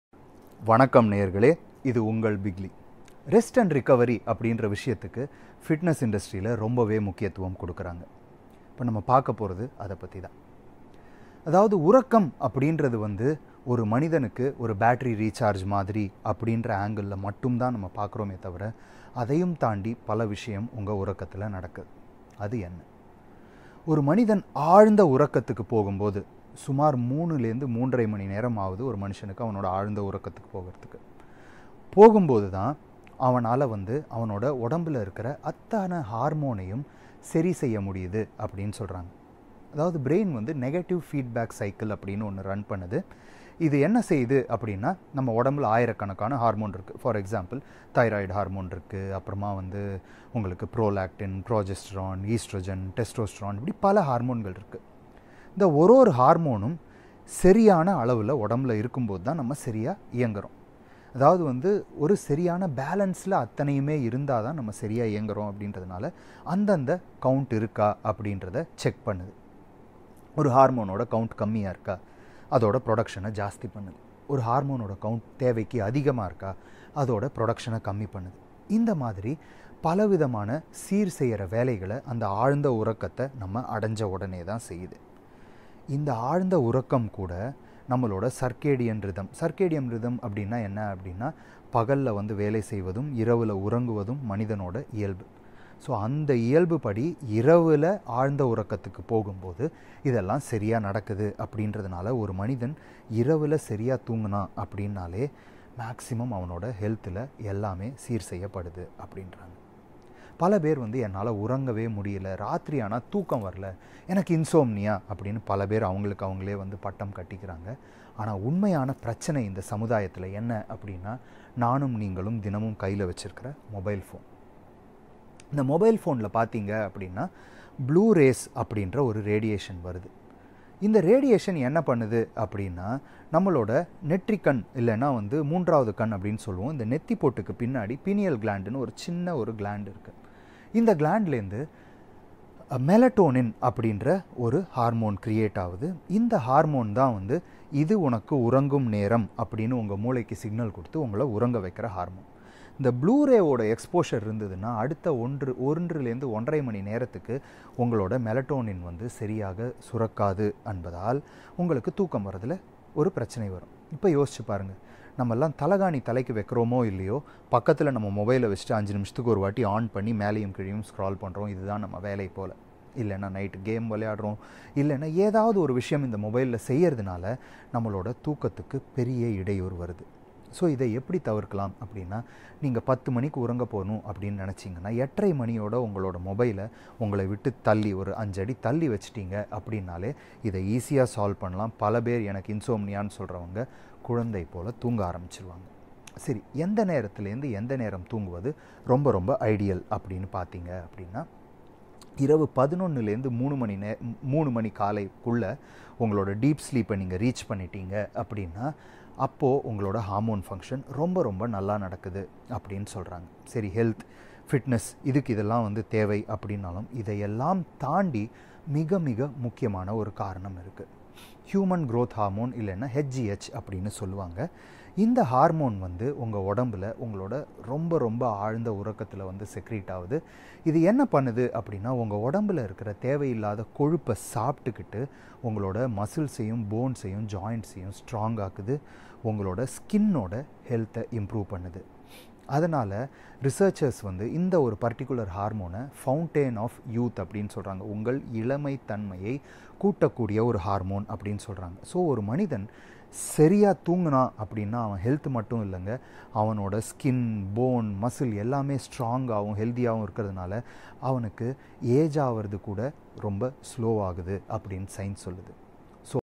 SLEEP - REST - RECOVERY - BIGLEE TAMIL
sleep is not just a recharge of battery - but a way to control hormones and overall health
see the full video to understand better